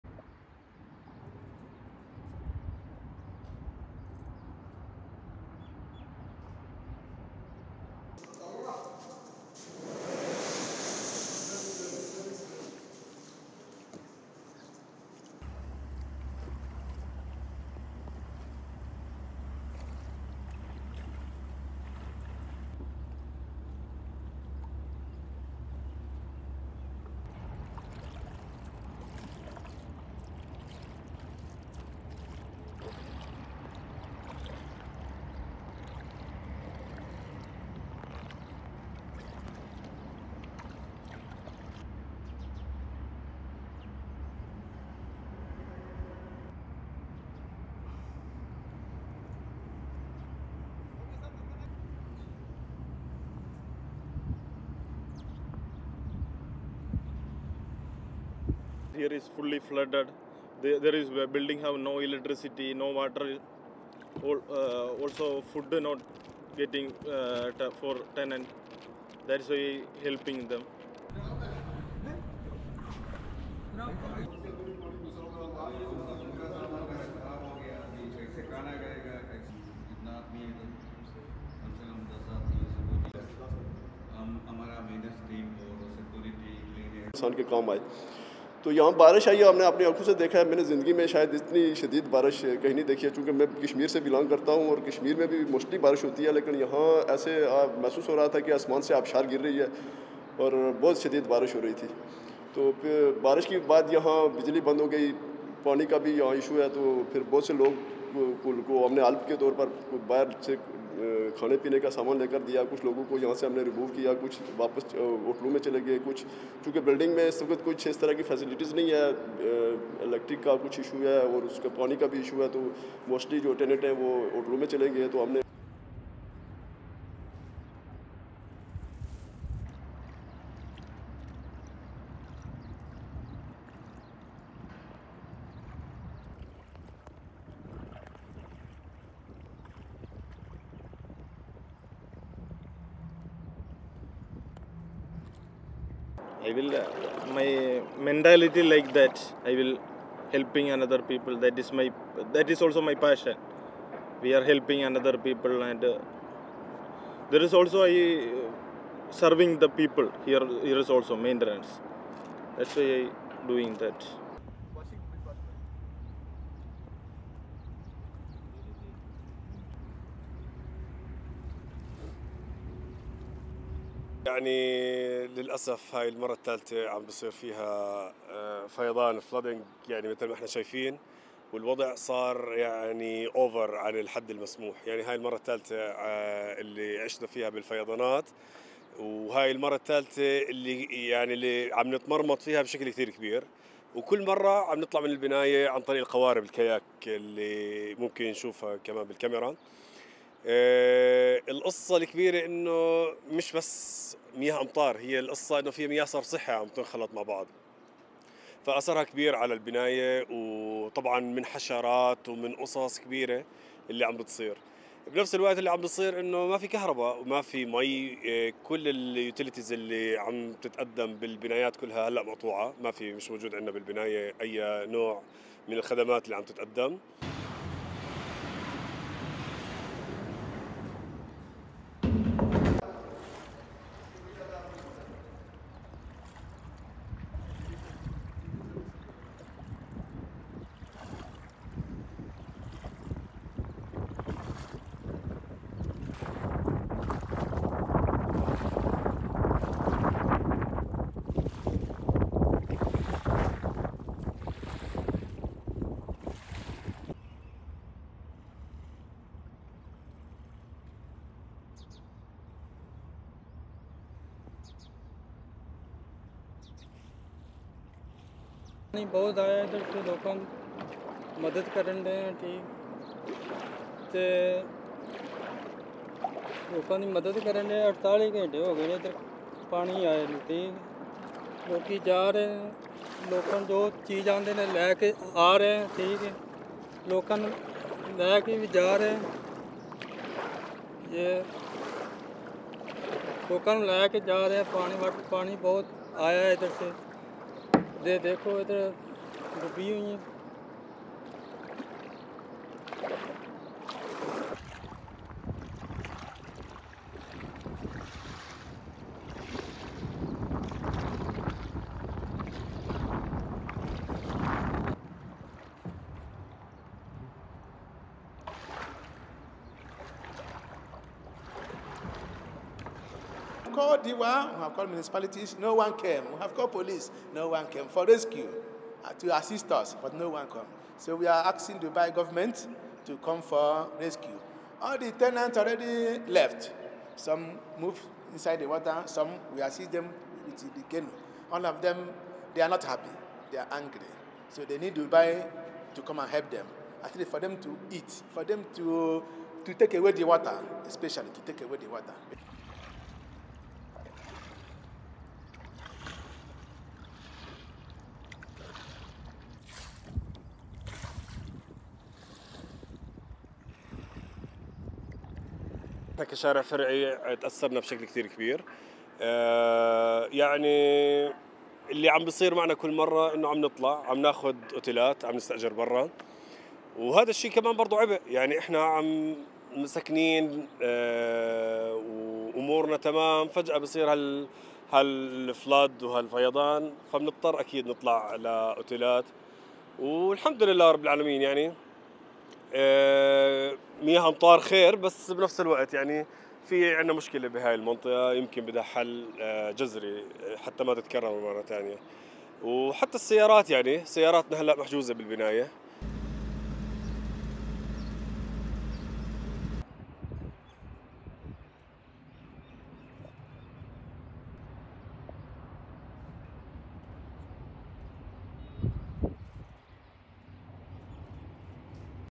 Volunteers use kayaks to help Dubai residents stranded by flood waters | ABS-CBN News
The storm, which first hit neighbouring Oman on Sunday, pounded the United Arab Emirates (UAE) on Tuesday, flooding roads and causing gridlock that lasted for hours as rainwater inundated homes and businesses. One person was reported dead in the UAE and 20 in Oman.